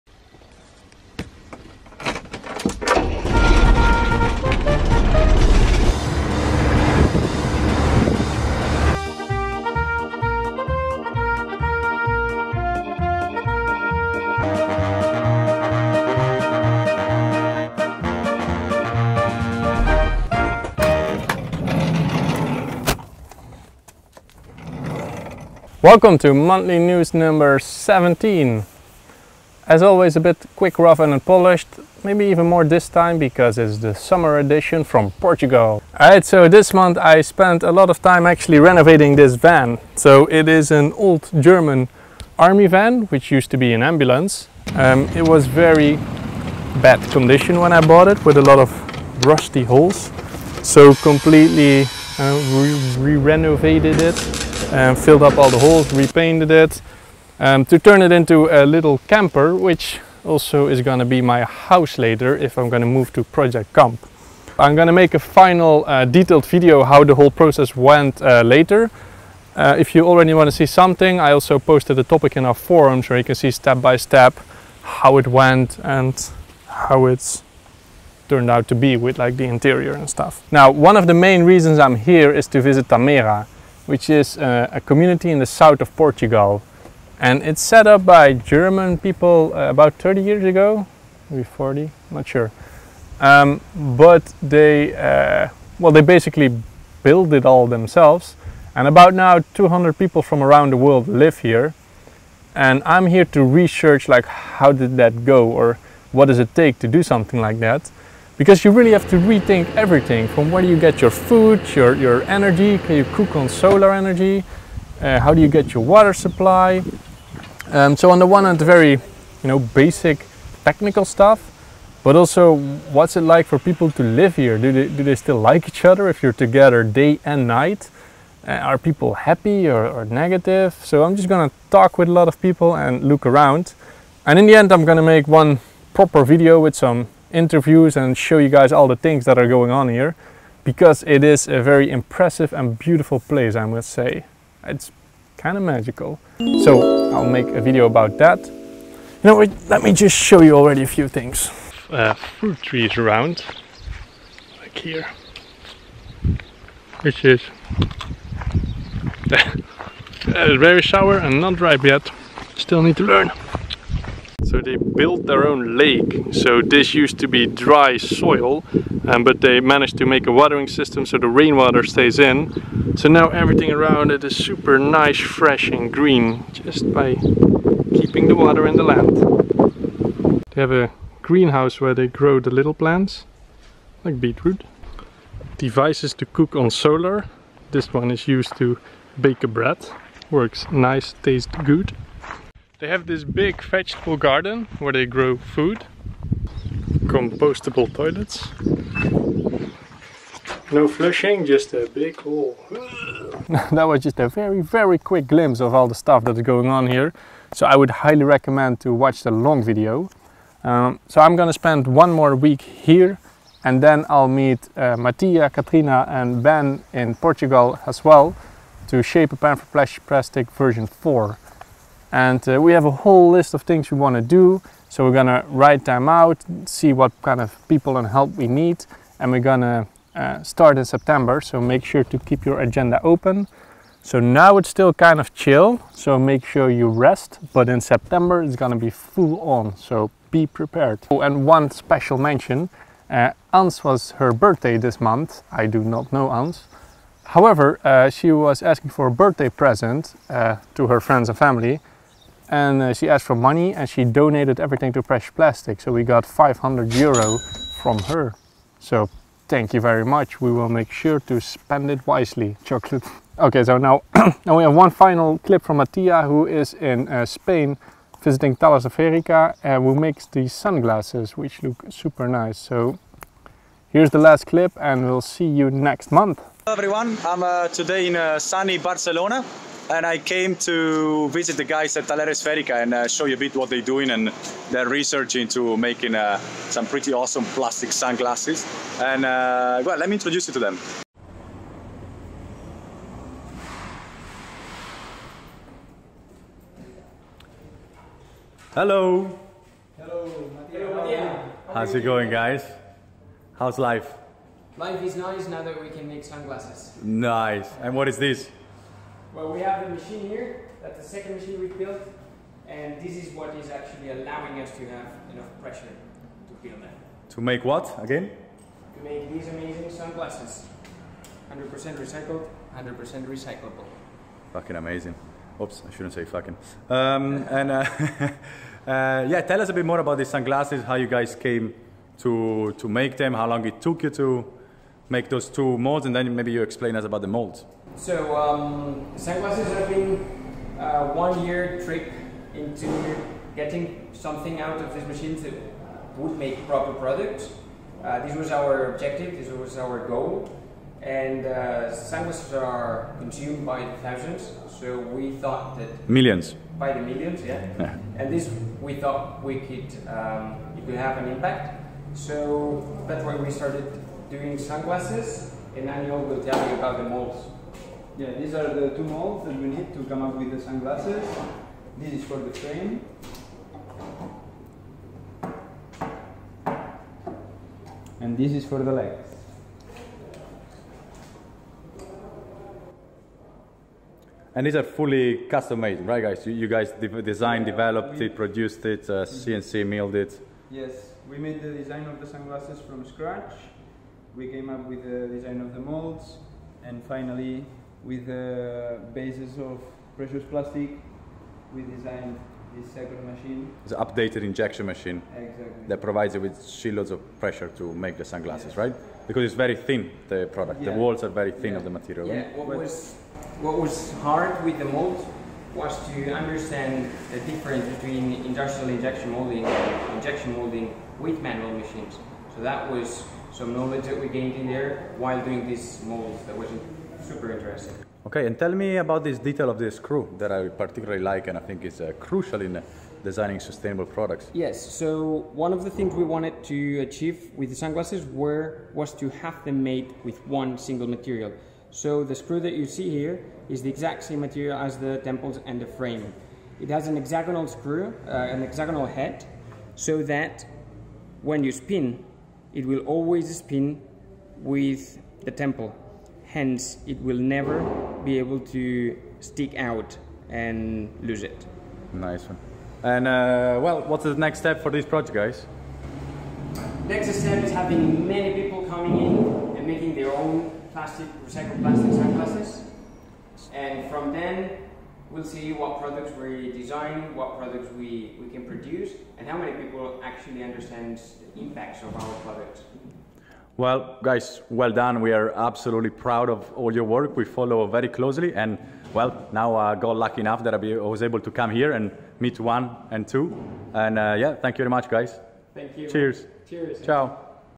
Monthly News #17 - Clean sunglasses from recycled plastic
Transformed an old german army ambulance to a camper and went to research Tamera. A beautiful community in the south of Portugal. And Mattia went to visit Tallers Esferica in spain to check out their super clean sunglasses from precious recycled plastic.